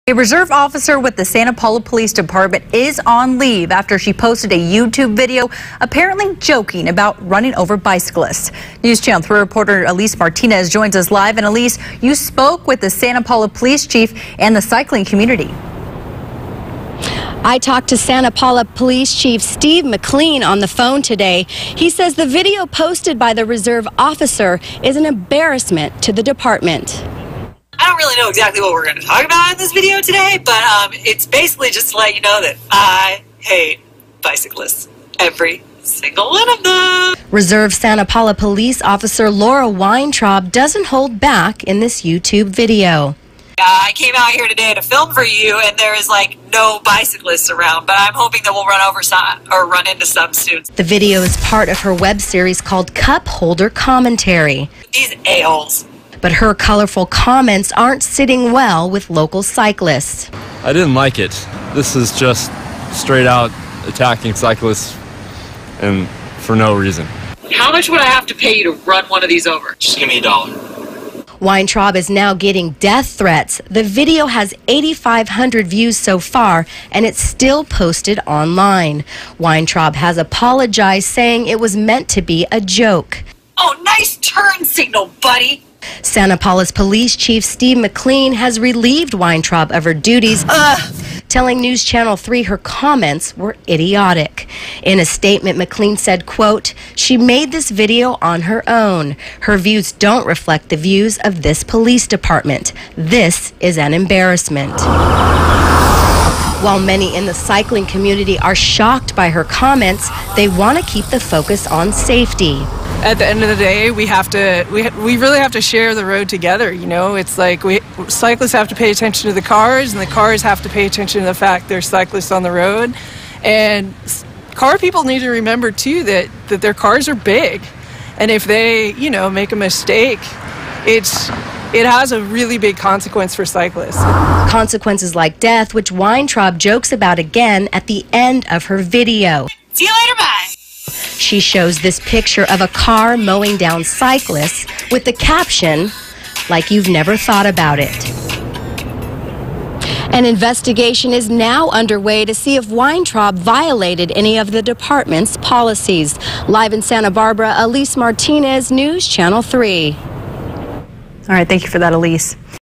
Reserve Officer On Leave After Posting Controversial Video
A reserve officer at the Santa Paula Police Department is on leave after posting video apparently joking about running over bicyclists